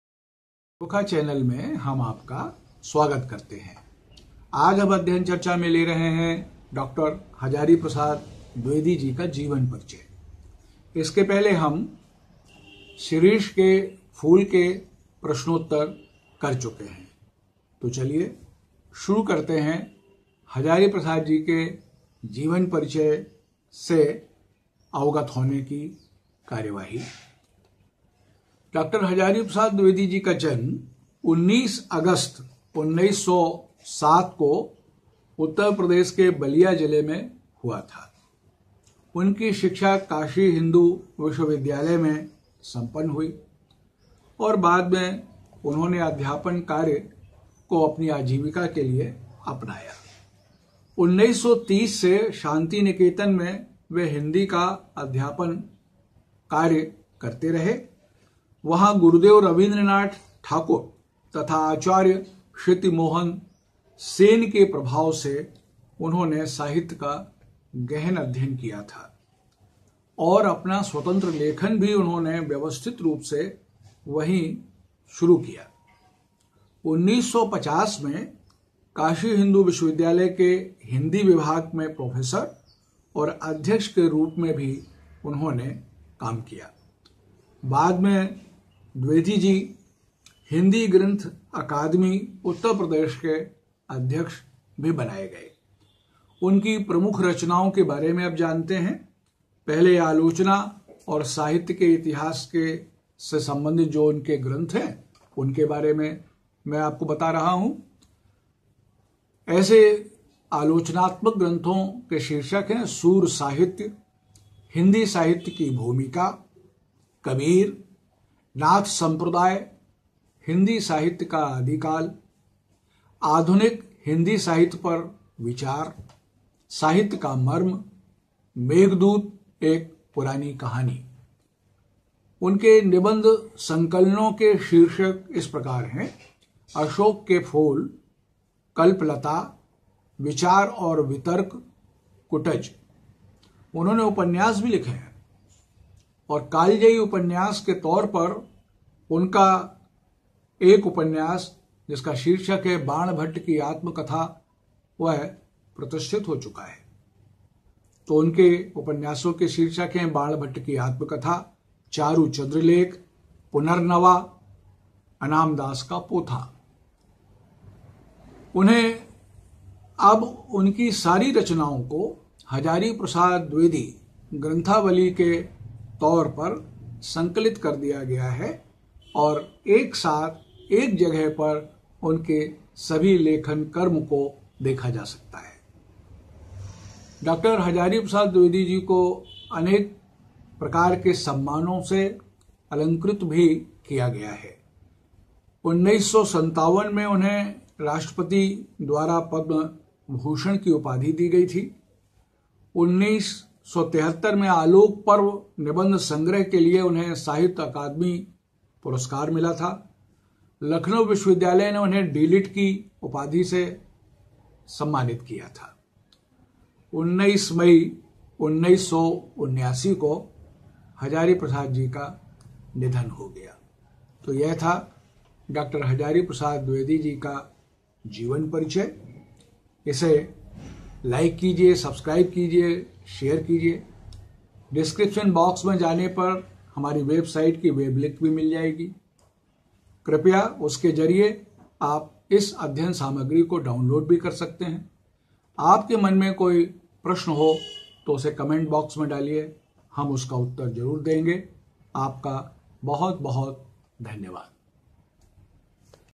hazari prasad dwivedi ka jeevan parichay hindi mai class 12 हजारी प्रसाद द्विवेदी का जीवन परिचय
Learn about Hajari Prasad Dwivedi biography his books and awards.
हजारी प्रसाद द्विवेदी का जीवन परिचय।

जन्म : 19 अगस्त 1907 ई० को उत्तर प्रदेश के बलिया जिले में।
शिक्षा : काशी हिन्दू विश्वविद्यालय से।
अध्यापन कार्य :
1930 से शांति निकेतन में हिन्दी का अध्यापन प्रारम्भ किया। वहाँ गुरुदेव रवींद्रनाथ ठाकुर तथा आचार्य क्षितिमोहन
सेन के प्रभाव से साहित्य का गहन अध्ययन किया तथा अपना स्वतंत्र लेखन भी व्यवस्थित रूप से आरंभ किया।
1950 में काशी हिन्दू विश्वविद्यालय के हिंदी विभाग में प्रोफेसर और अध्यक्ष के रूप में कार्यभार ग्रहण किया।